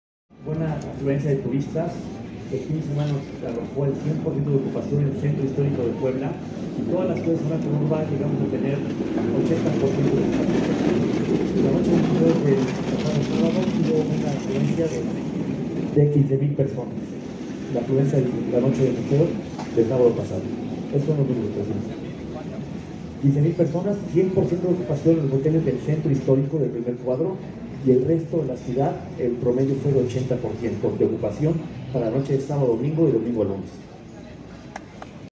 Durante el puente, se registra una ocupación hotelera del 100% en la zona del Centro Histórico
El titular de Economía y Turismo, Alejandro Cañedo aseguró que durante este puente pasado, se registró una ocupación hotelera del 100% en la zona del Centro Histórico y de un 80% en el resto de la ciudad.